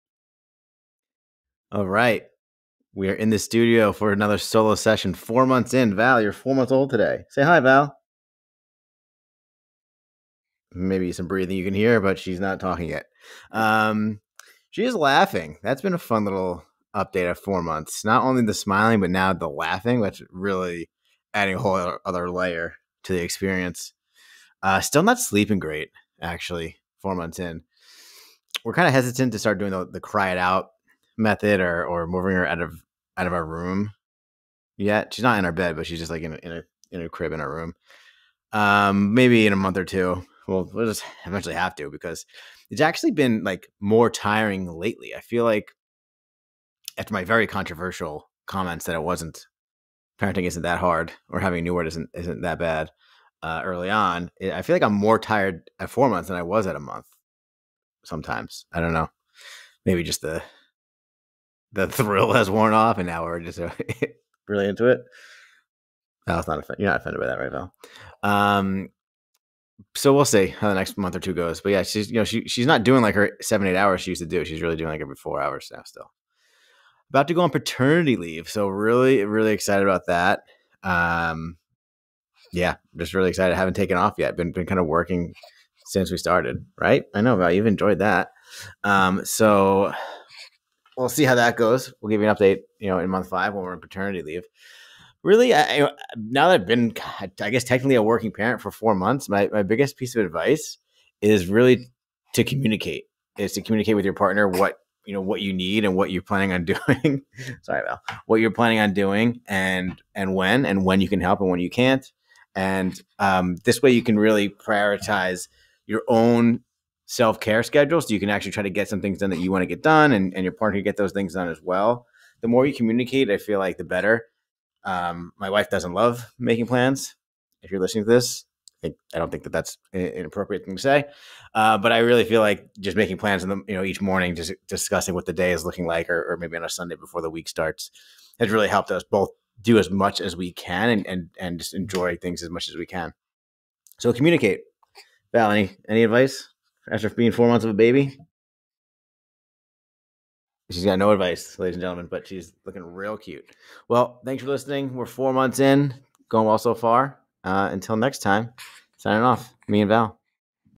Solo Episode | Month 4 of fatherhood
Four months into being a dad. Here's a quick update on how it's going so far. Special guest baby Val!